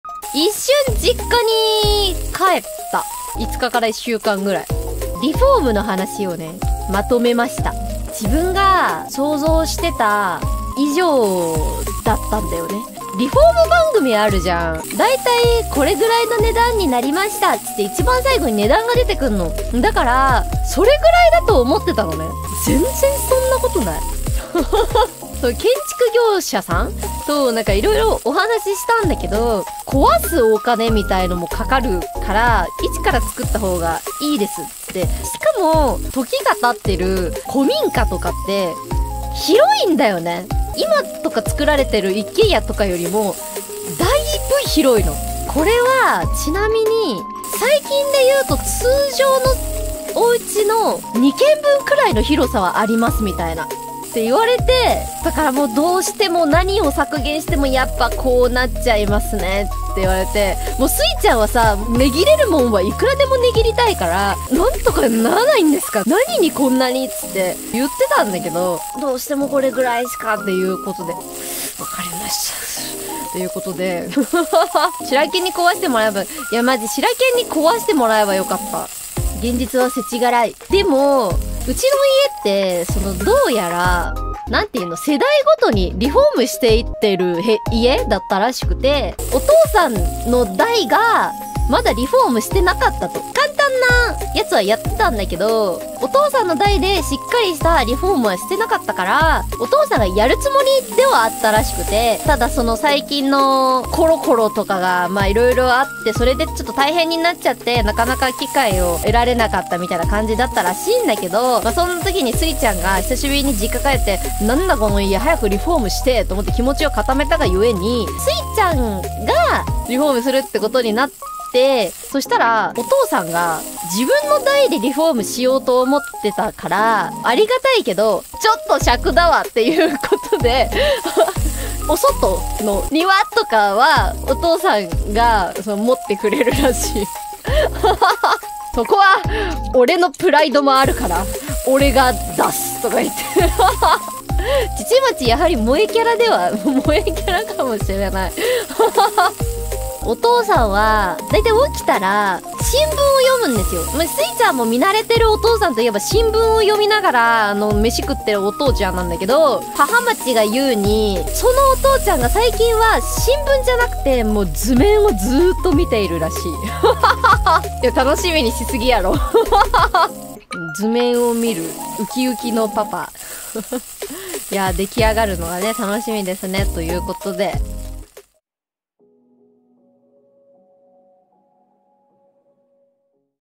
Splitting Costs With Papa-machi (Hoshimachi Suisei / Hololive) [Eng Subs]
Hoshimachi Suisei talks about splitting the house renovation cost with her father...

Thanks to Vee for the TL check!